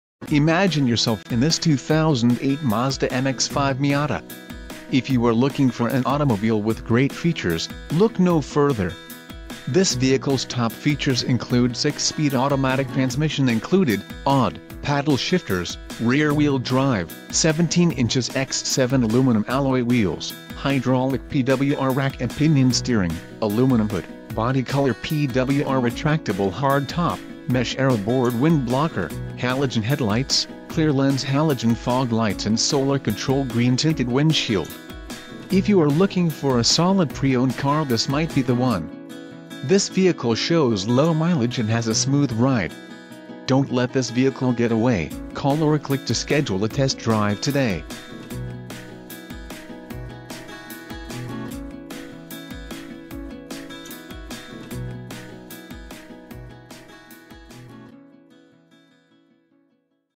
2008 Mazda MX-5 Miata Grand Touring - Faulkner Mazda Trev...
Superb Condition, GREAT MILES 54,508! Heated Leather Seats, Multi-CD Changer, Convertible Hardtop, ALL-WEATHER FLOOR MATS, Aluminum Wheels, Premium Sound System, Just PA state inspected ! SEE MORE!KEY FEATURES INCLUDE: Heated Driver Seat, Premium Sound System, Multi-CD Changer, Convertible Hardtop, Heated Seats, Heated Leather Seats. Remote Trunk Release, Keyless Entry, Steering Wheel Controls, Bucket Seats.OPTION PACKAGES: ALL-WEATHER FLOOR MATS. MX-5 Miata Grand Touring with Highland Green Mica exterior and Tan interior features a 4 Cylinder Engine with 158 HP at 6700 RPM*. Just PA state inspected !EXPERTS CONCLUDE: Edmunds.com's review says Miata is still unequaled when it comes to overall driving enjoyment and refinement.. Approx. Original Base Sticker Price: $28,200*.MORE ABOUT US: Come to Faulkner Mazda Test Drive the newest Mazda vehicles or Used Cars and Trucks.